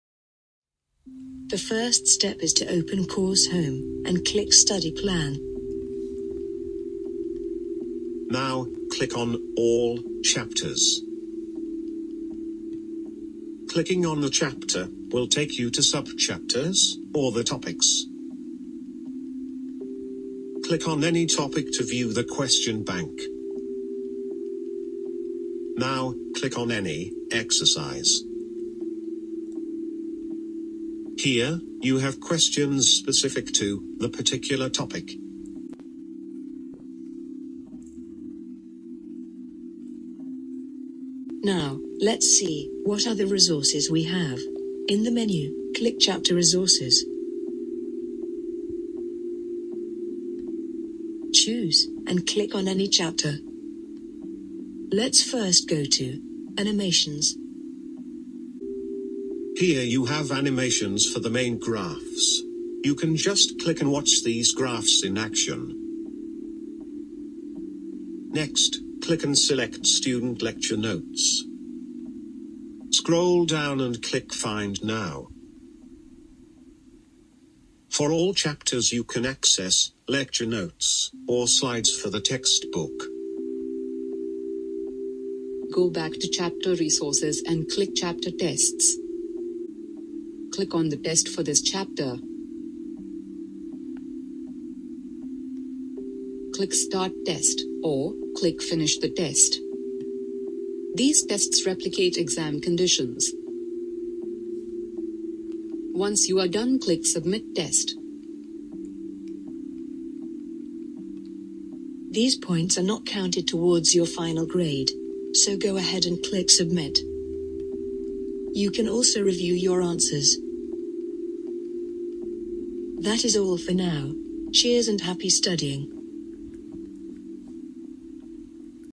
Mylab - Practice Questions & Student Resources
Pearson Mylab Economics
Student's Resources. How to access practice questions etc. in the Pearson learning management system.
This video is for Students.